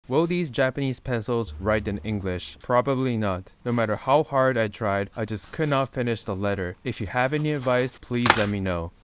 Will these Japanese pencils write in English?
Wooden pencils from Japan can have a hard time to write.